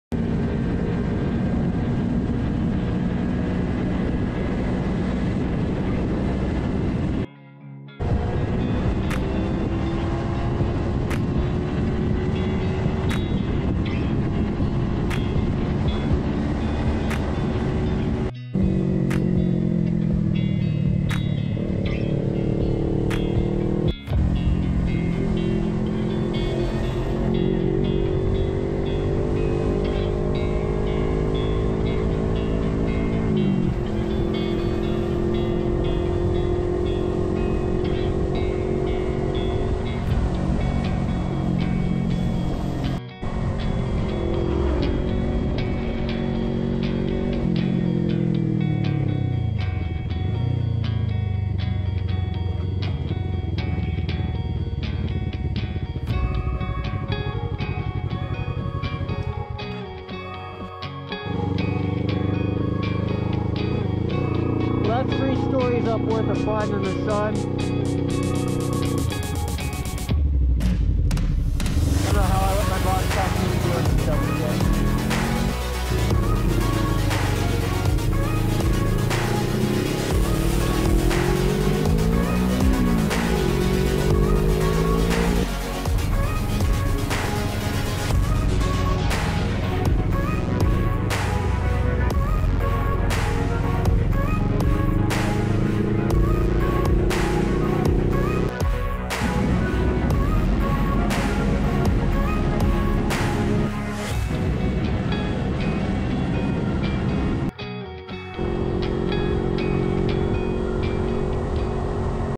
Trippin 250 Tuesdays Fun Rip
idk how I let my boss talk me in to gettin on top of this house...